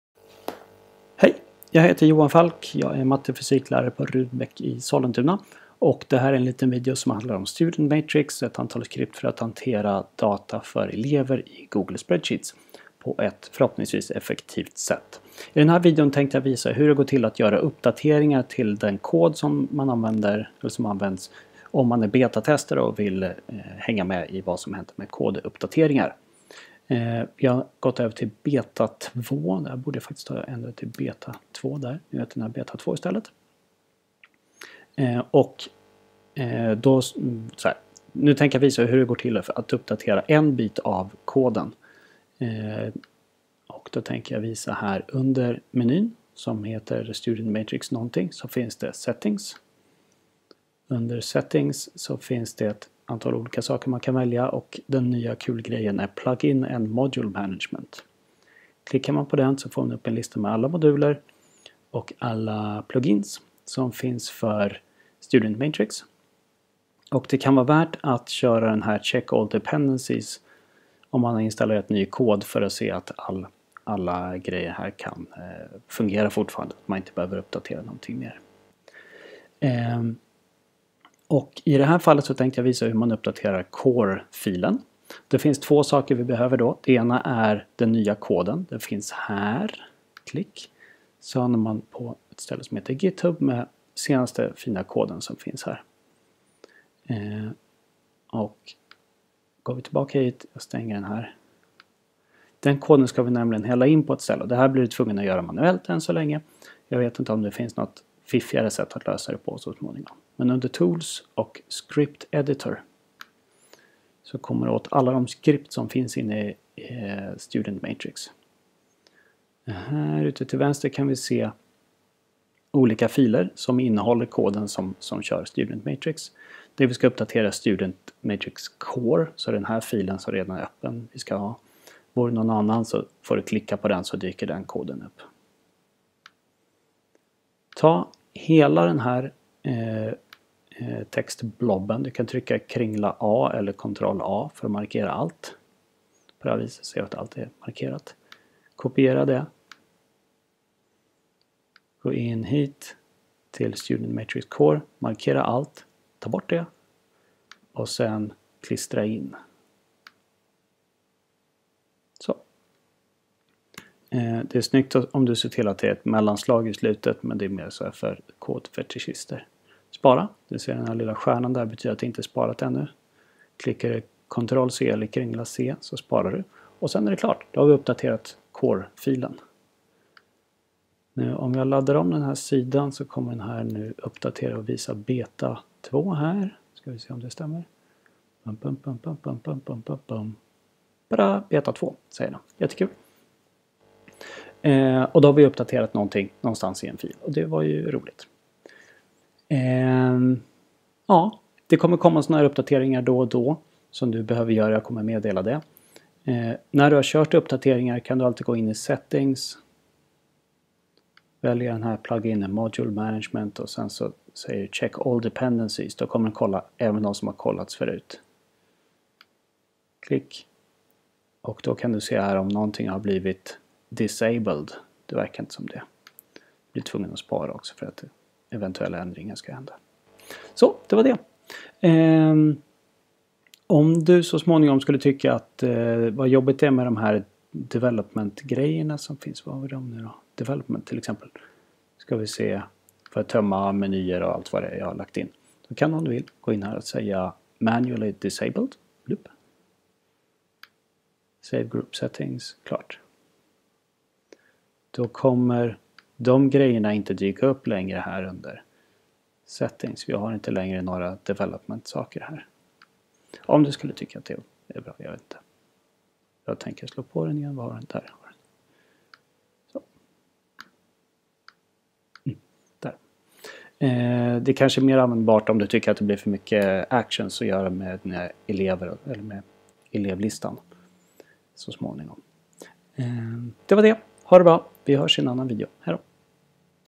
StudentMatrix 3.0-beta2: uppdatera kod
Den här videon visar hur det går till att uppdatera kod för dig som betatestar StudentMatrix 3.